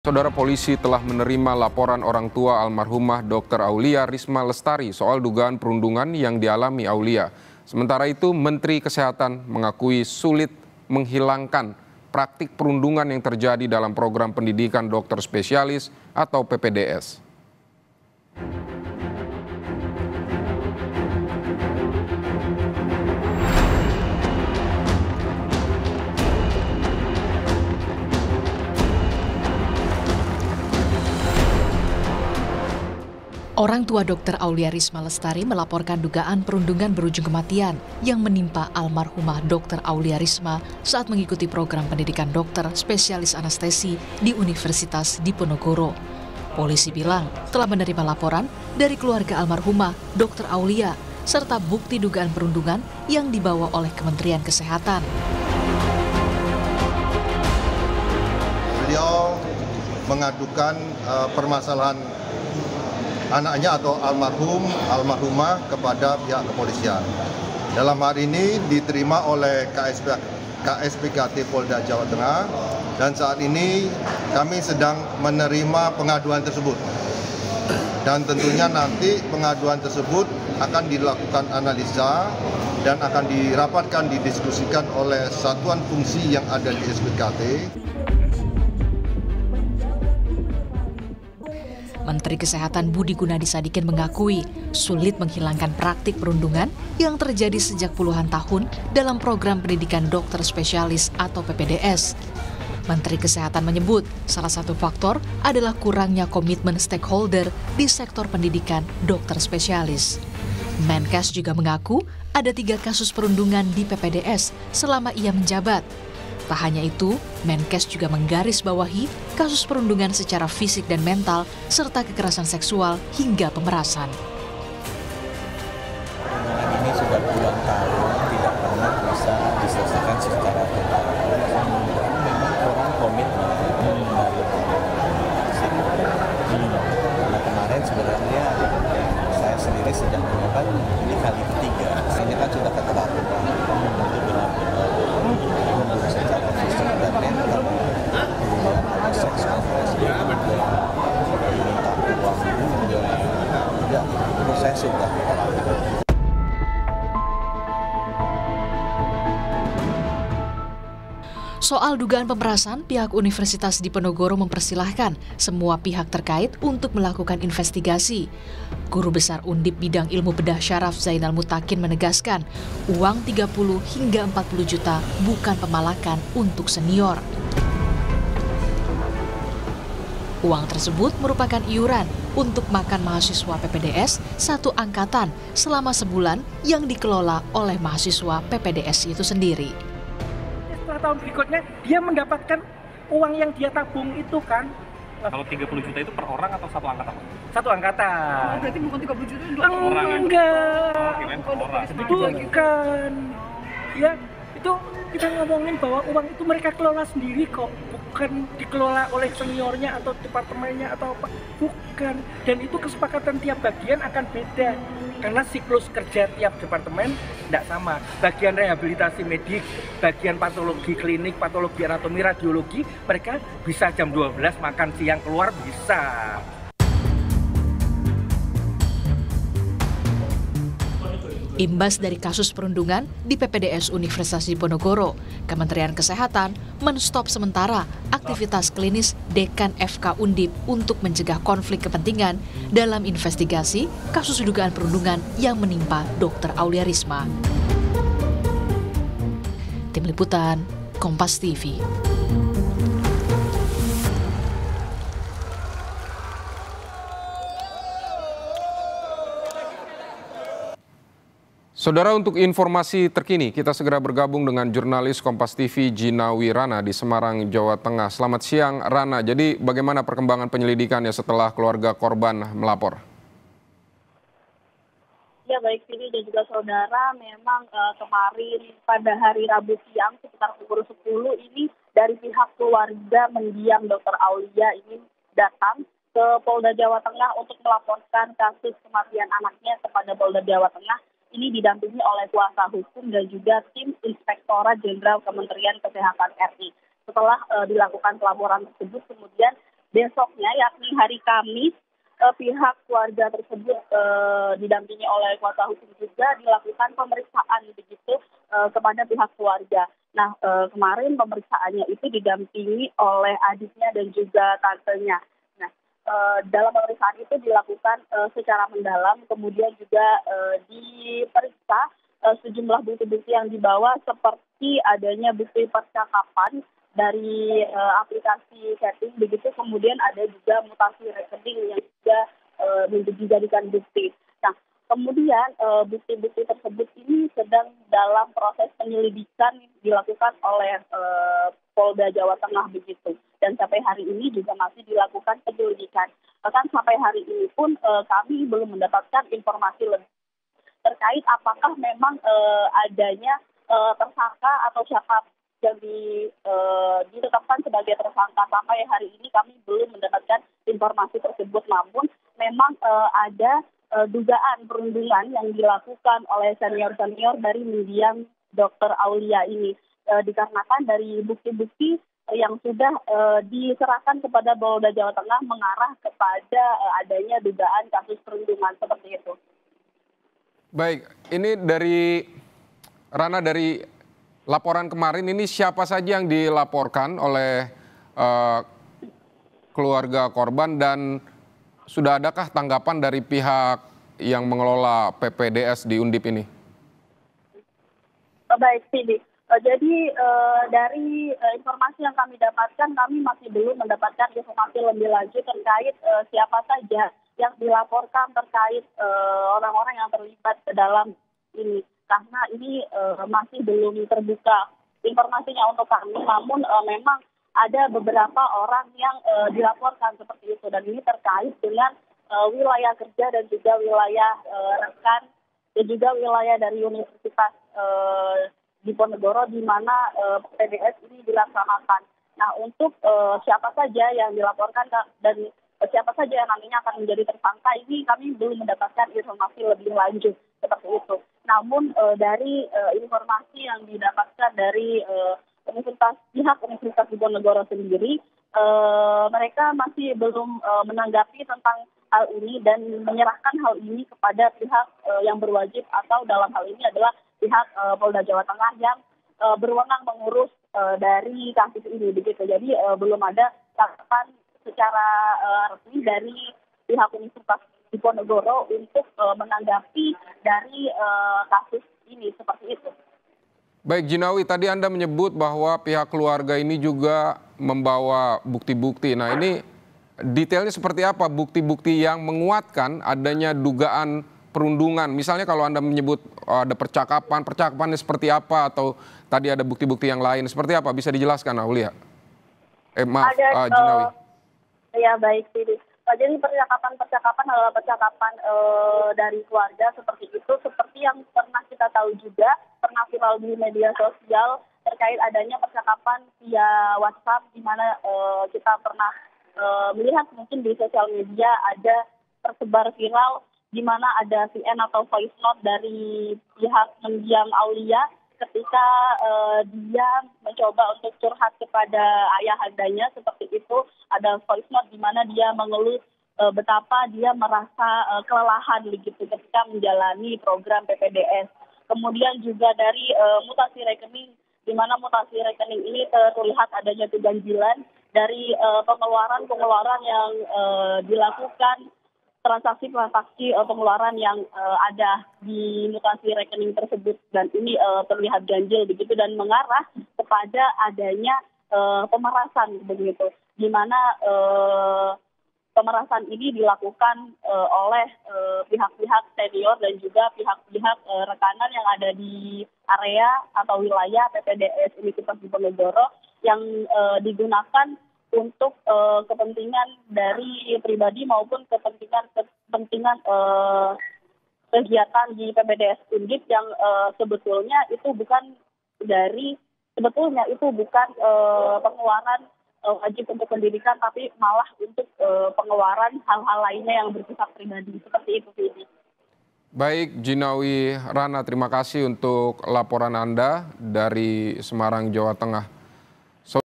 Polisi Selidiki Dugaan Perundungan Dokter PPDS, Menkes Sebut Bullying Sulit Dihilangkan
KOMPAS.TV - Polisi Selidiki Dugaan Perundungan Dokter PPDS, Menkes Sebut Bullying Sulit Dihilangkan

Kompas TV
Independen | Terpercaya